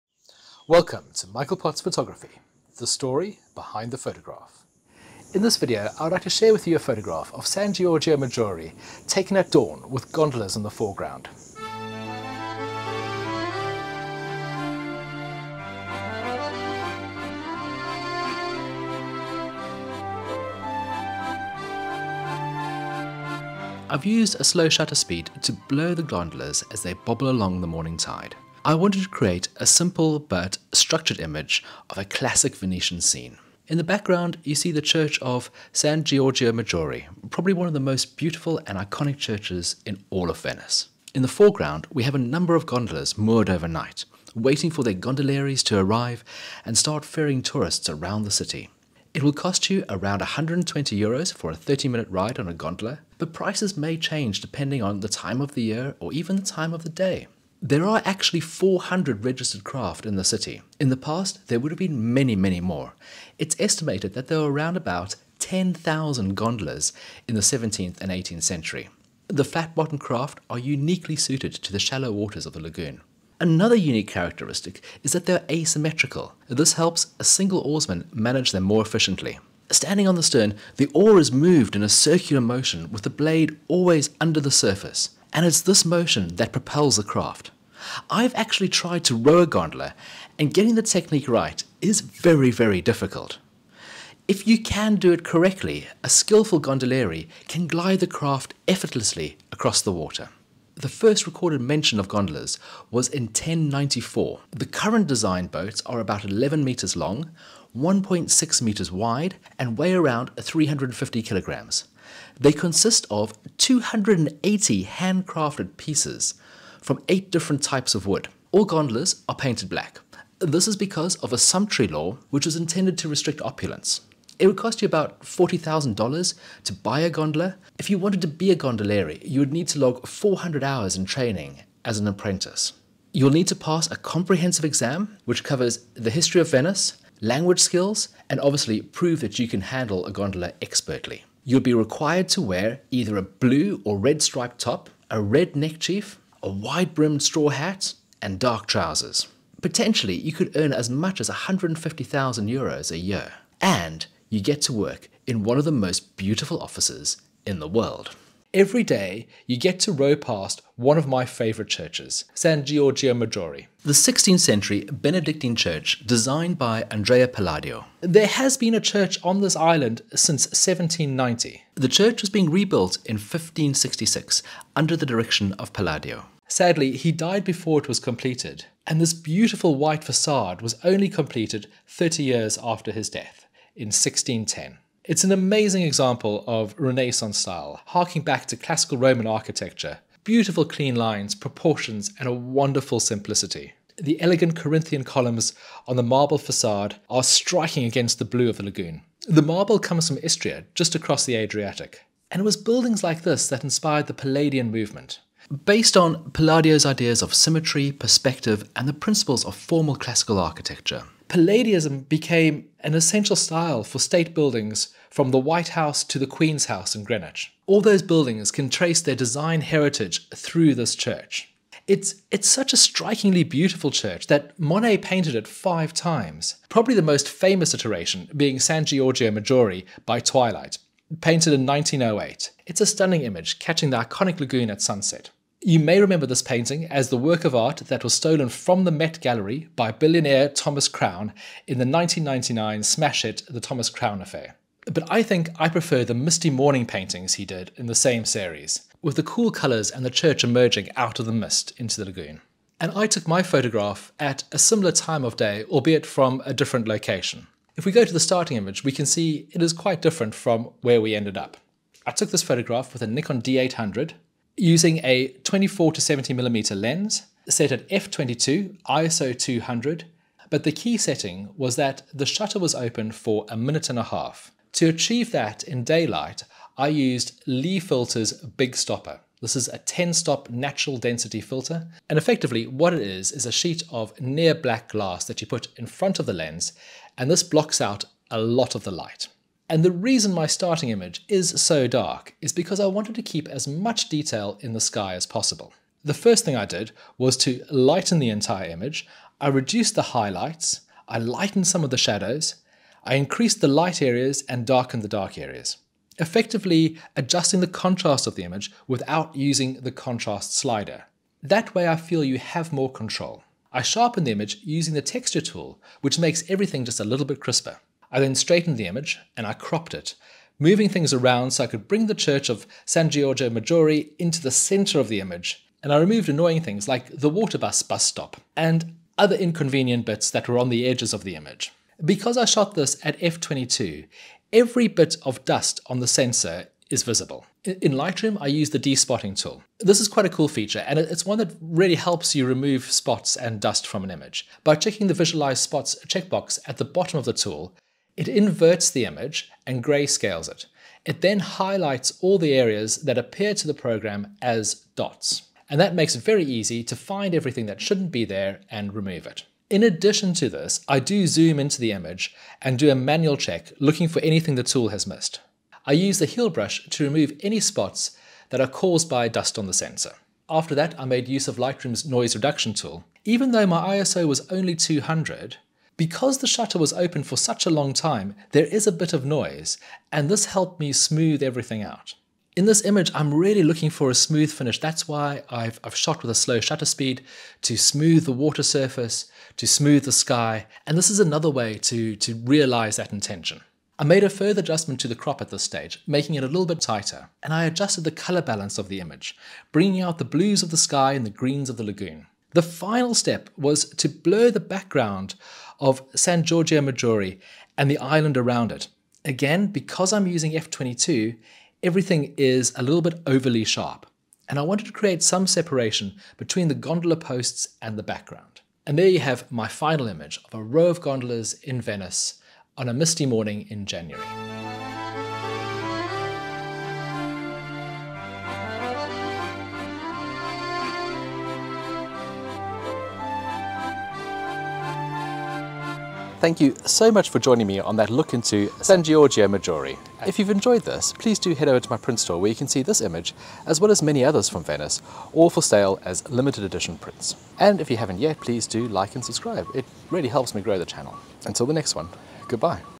San Giorgio Maggiore at Dawn
The story behind this photograph of San Giorgio Maggiore at Dawn. This is a slow shutter speed photograph of Venetian Gondolas with Andrea Palladio's church in the background.  This video explores the history of gondolas in Venice and Palladian architecture.